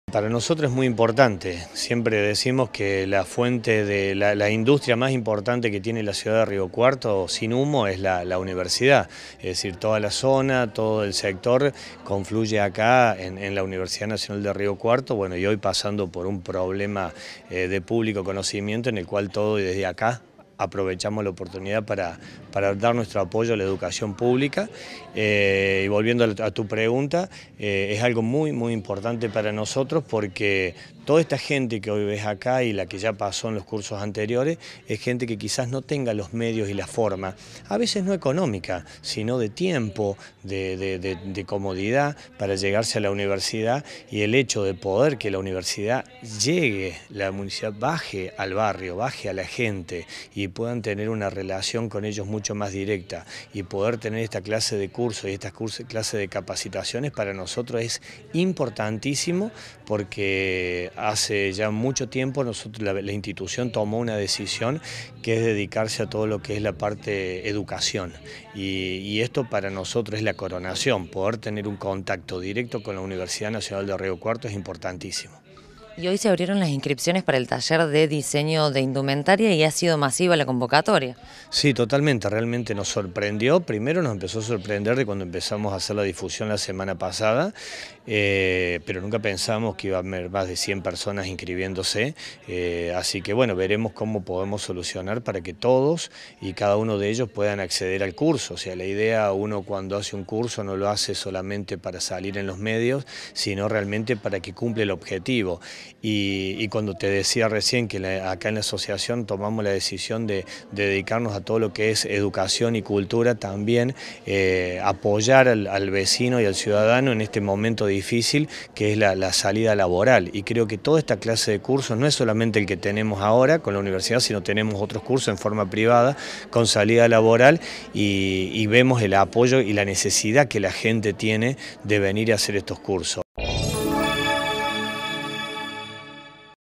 UNIVERSIDAD BARRIAL - LEONARDO GRANGETTO
MÁS DE 70 INSCRIPTOS EN EL TALLER DE DISEÑO Y CONFECCIÓN DE INDUMENTARIA
Facebook.com/uniriotv
Twitter.com/uniriotv